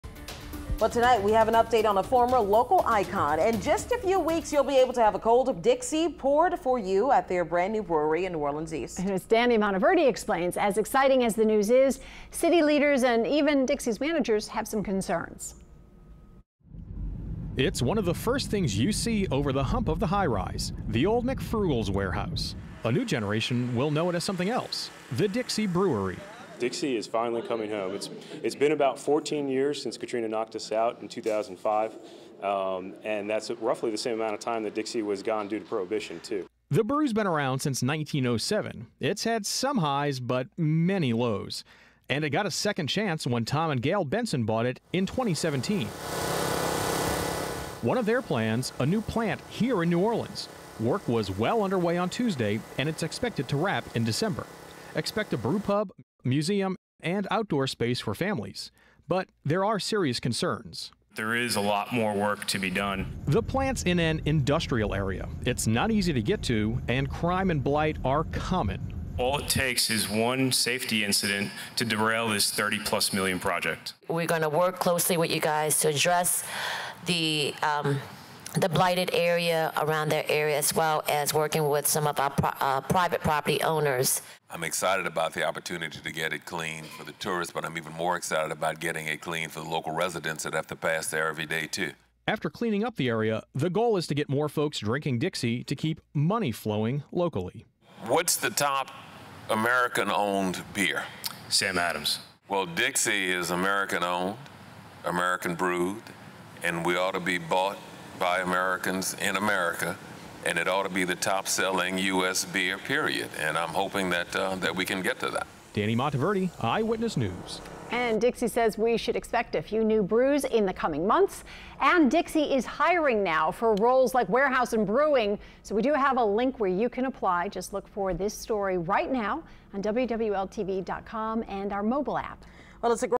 New Dixie Brewery in New Orleans East prepares to open its doors
By December, the beer will be brewed at the new plant. Birch said he hopes doors will open to the public by January 2020.